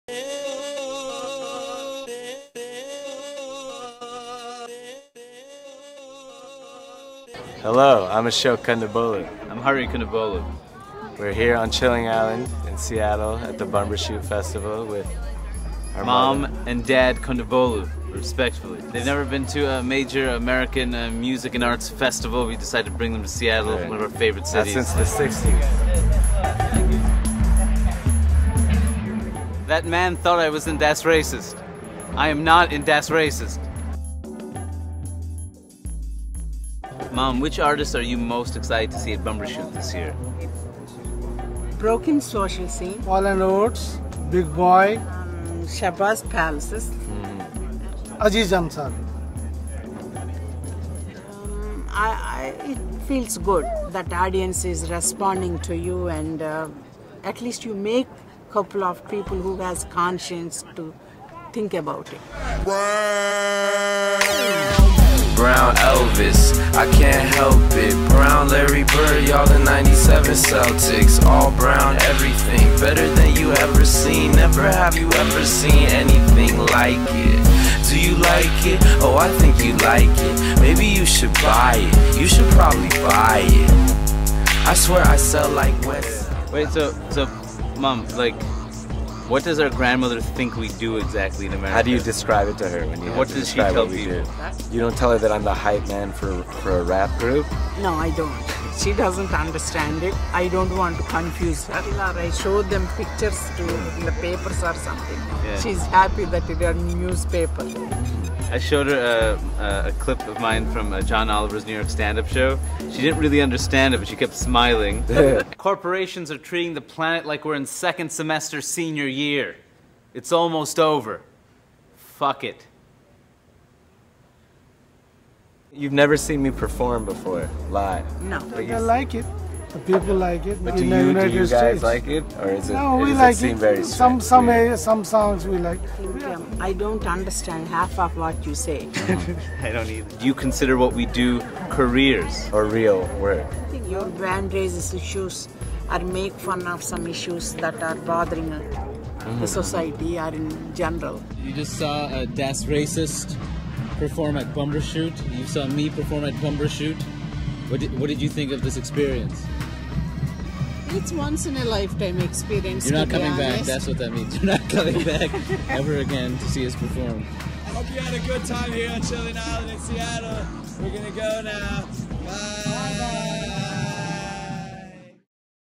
Backstage Pass: Das Racist
Watch Das Racist drag their parents to a music festival. Mom and Pop Kondabolu are big fans of Broken Social Scene!